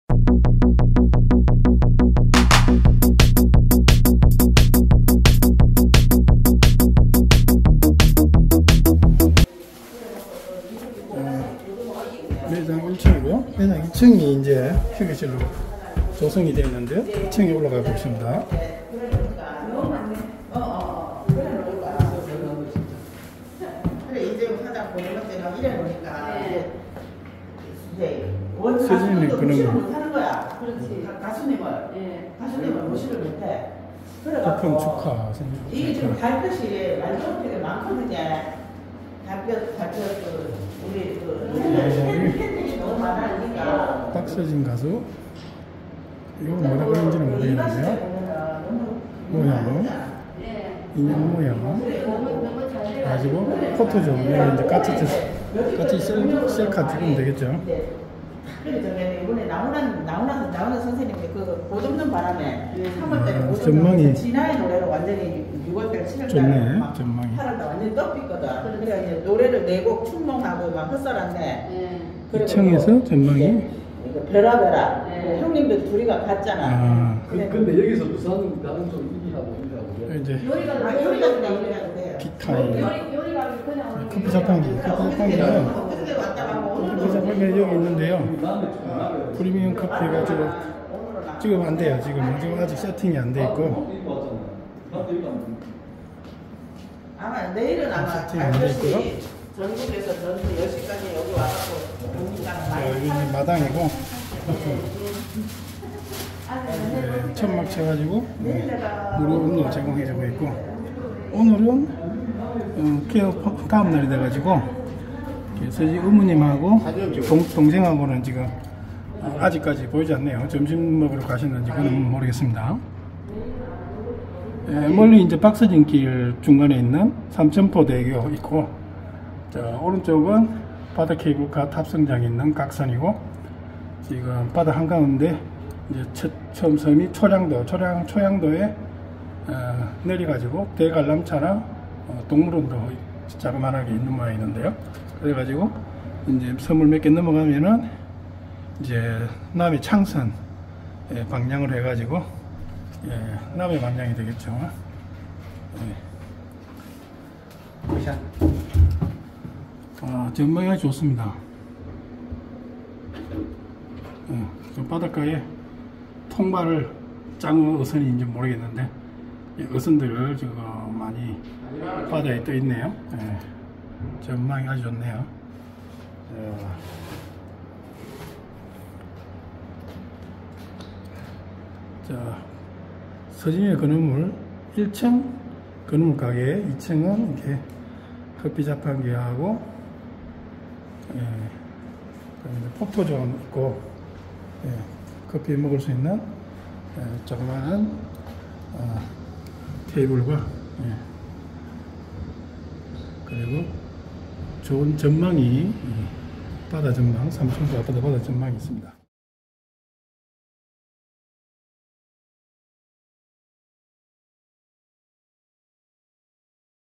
서진이네 개업. 대박! 2층 박서진포토존, 자판기휴게실
서진이네건어물 오픈 첫주말 풍경. 2층 박서진포토존, 사천바다 조망
박서진 어머님 오후에 오셨다가 박서진가수 사천집에 와서 쉬고 있다하여 다시 집에 들리러 가신 모양이네요.